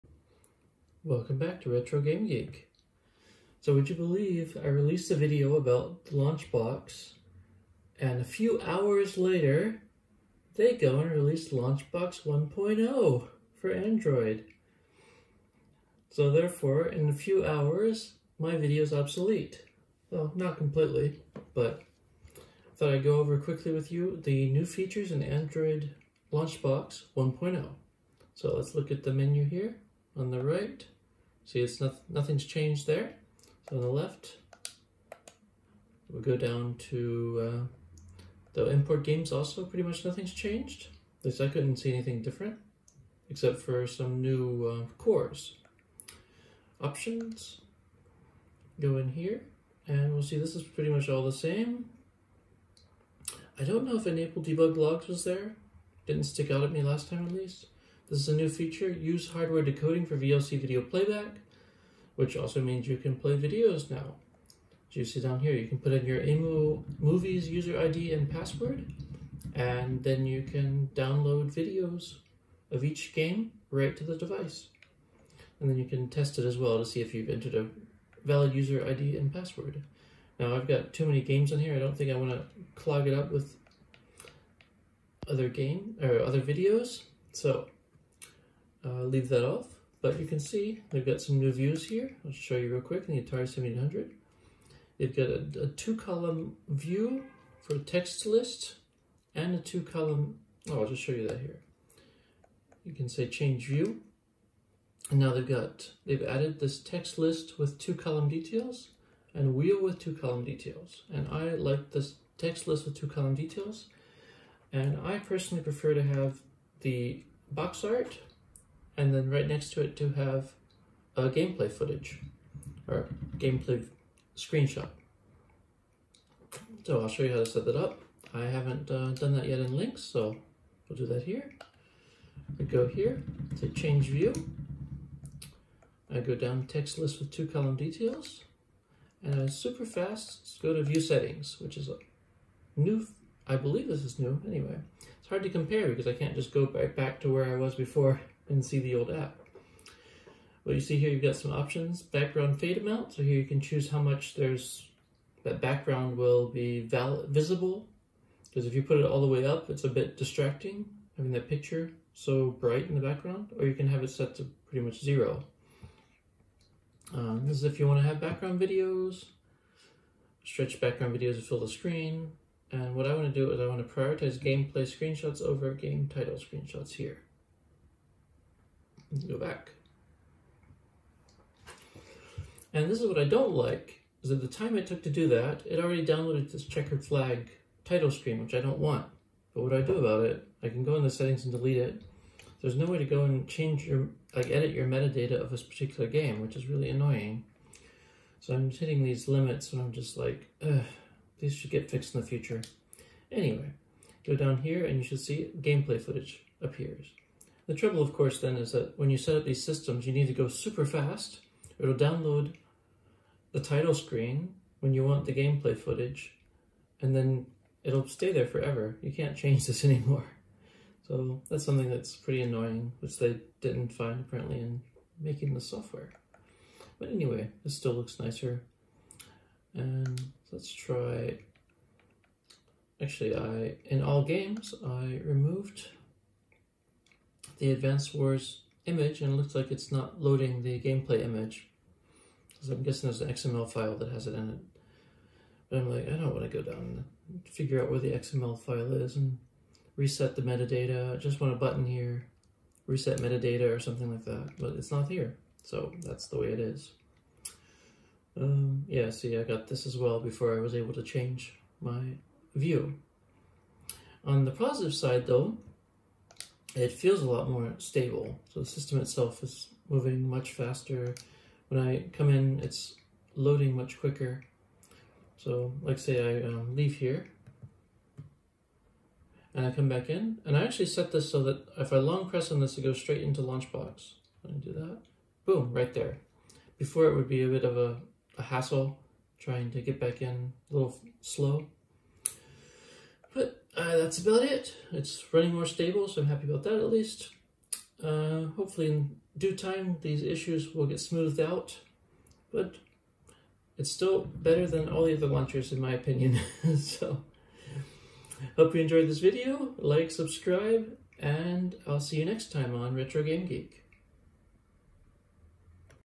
LaunchBox 1.0 update today! What’s new? (Android)
LaunchBox 1.0 update is out! It fixed my Atari 5200 problem, but my BSNES issue still exists (when I autoload games from saves). At least it loads a lot faster and is more stable! I'm not a fan of them moving All Games to the far right though. :-/ Hear what I like and what frustrates me about LaunchBox 1.0 for Android.